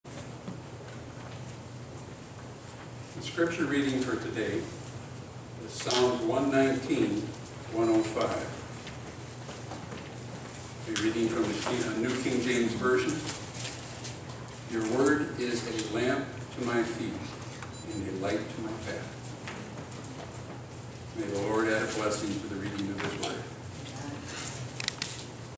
NSBSDA - Scripture Reading - April 13, 2019
Presented By: Marty
Scripture: Psalm 119:105
New Smyrna Beach Seventh Day Adventist Church